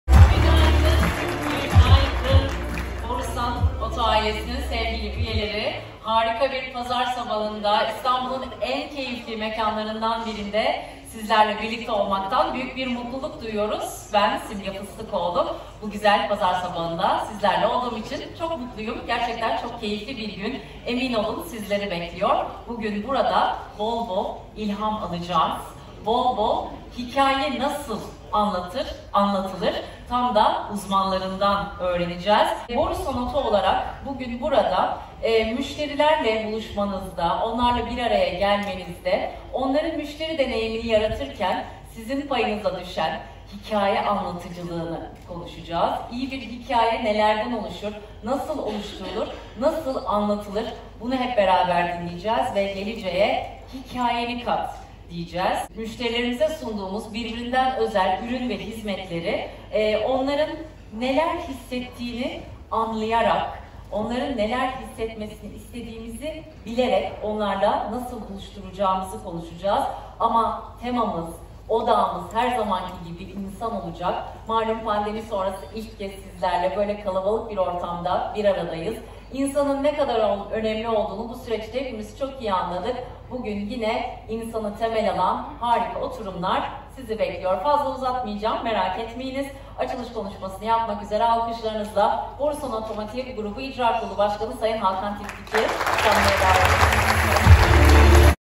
Borusan Oto Müşteri Zirvesi - 26 Eylül 2021
Borusan Oto Müşteri Zirvesi Simge Fıstıkoğlu’nun sunumuyla Kemer Country / Orman Evinde gerçekleştirildi.